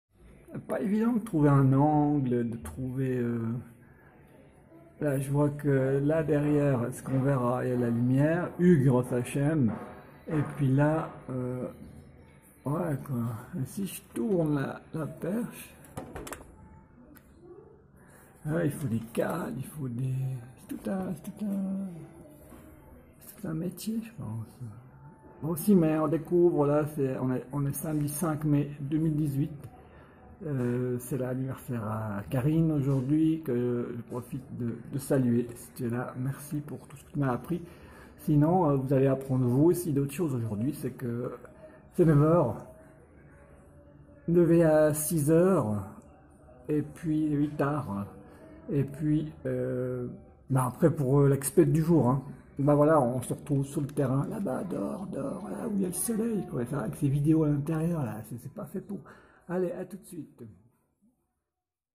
Épisode du 5 mai 2018 : Présentation de l'Excursion du jour à Rishikesh, Nord de l'Inde
Scènes  de  Vie  ici aux  alentours  de  Rishikesh.  Vidéo du  5  mai  2018

Au  Menu  du  Jour :

Venez vous aussi  découvrir ,  voyager et  vivre en  Inde en vous  offrant les  prodiges  qui vous  attendent  encore  dans  votre  évolution.